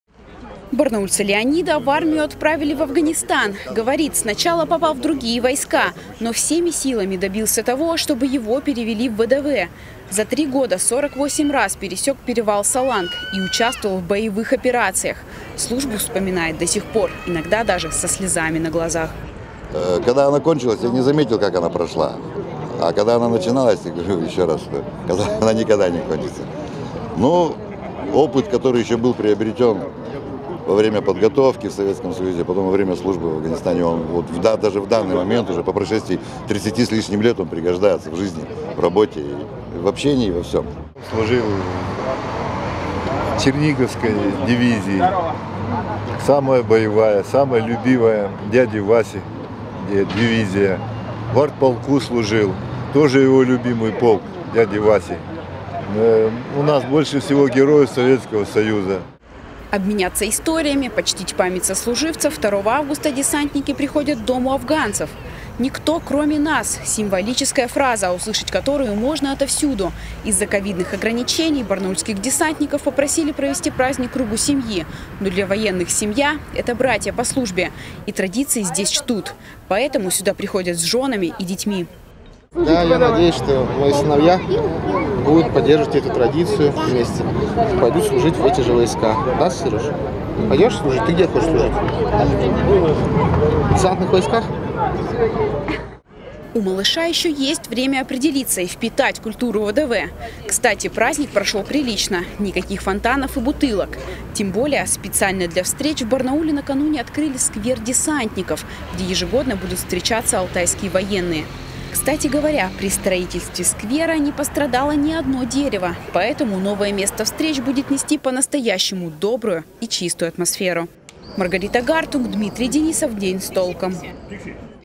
91-ю годовщину со дня образования сегодня отмечают Воздушно-десантные войска
91-ю годовщину со дня образования сегодня отмечают Воздушно-десантные войска. За всю свою длинную историю ВДВ прошли длинный путь от маленьких авиадесантных отрядов до отдельного рода войск, которые называют элитой российских Вооруженных сил. Как десантники отметили свой праздник в условиях ковидных ограничений, расскажет Маргарита Гартунг